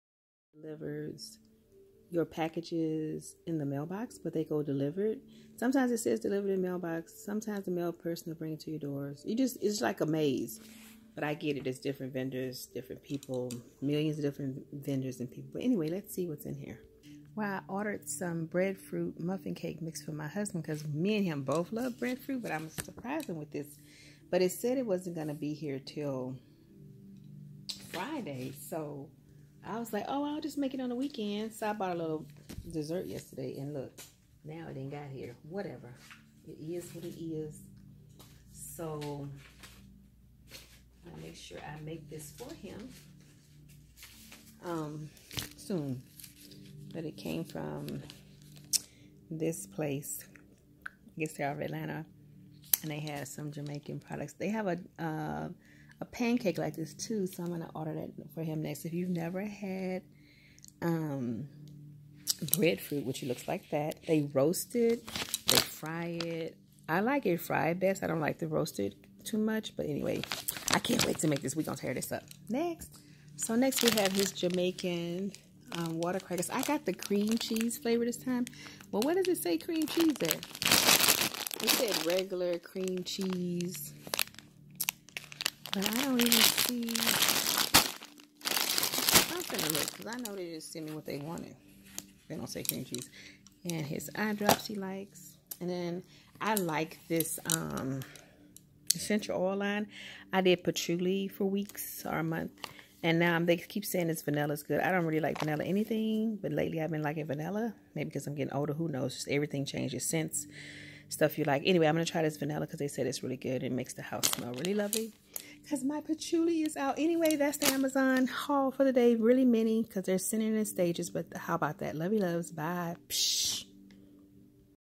Mini Amazon Haul
Hey Ya'll, Hey Ya'll!
I'm sharing my small Amazon haul, Ya'll!
Breadfruit muffin mix, come on, if you've never had breadfruit, where have you been? Originally from Jamaica.
Jamaican crackers, I loveeeeeee this essential oil line, trying the vanilla sent now. My hubby gotta have his Visine eyedrops.

Isaiah 43:19+